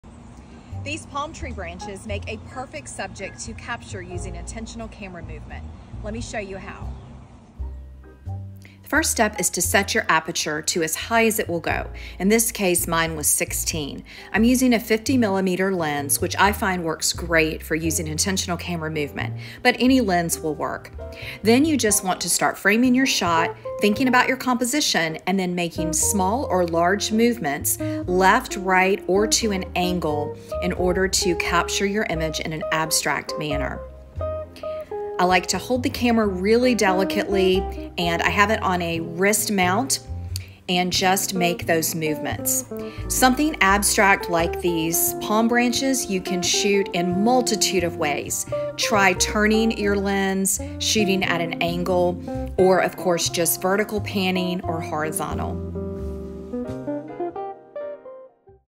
ICM garden photography|Abstract Photography
@ creative photography with Lori shares a quick ICm photo shoot with tips to capture an abstract